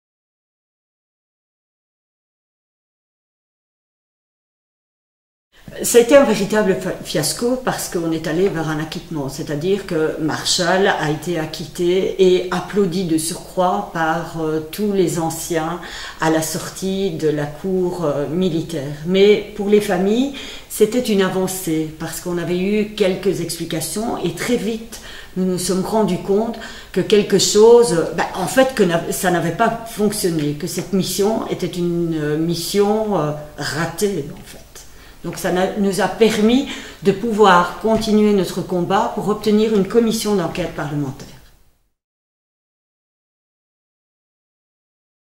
"Rwanda 94. Comprendre l'incompréhensible" Témoignage de Martine Debatty (2/2)
« Rwanda 94. Comprendre l’incompréhensible » est une exposition réalisée par la Province de Hainaut (Hainaut Mémoire), les Territoires de la Mémoire asbl et le War Heritage Institute, avec le soutien de la Fédération Wallonie-Bruxelles (cellule Démocratie ou barbarie).